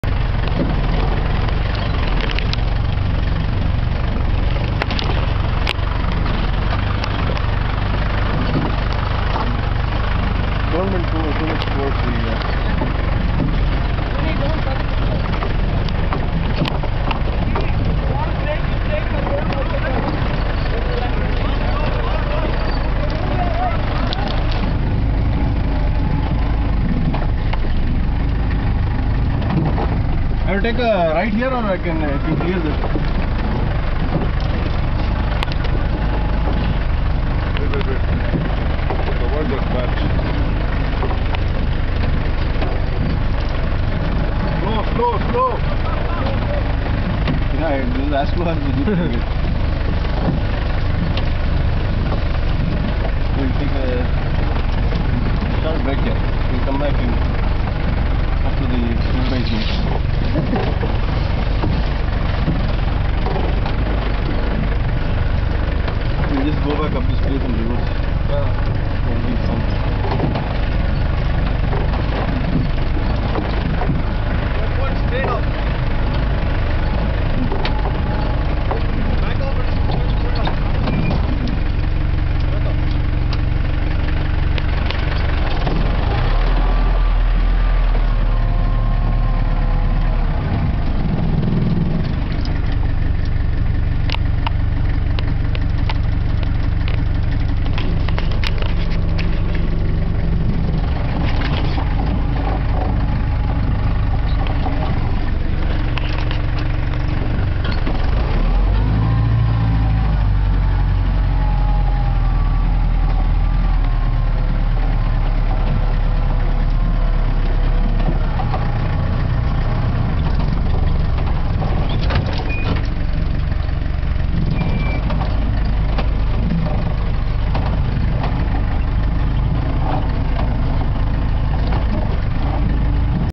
Offthrill action from inside!
Some offroading action captured from the passengers seat